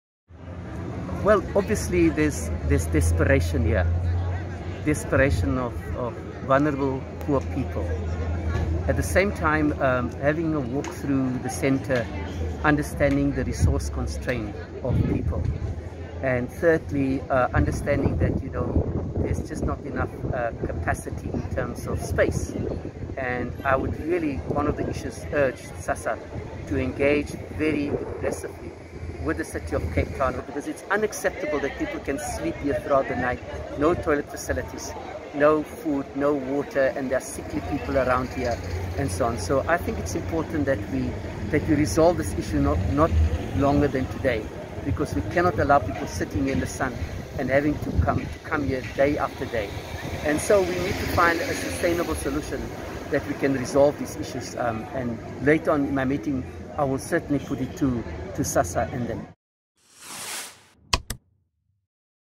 WATCH | SAHRC urges Sassa and City of Cape Town to work together to ease queues
The South African Human Rights Commission (SAHRC) has urged the South African Social Services Agency (Sassa) to get the City of Cape Town to release community halls for use as satellite offices.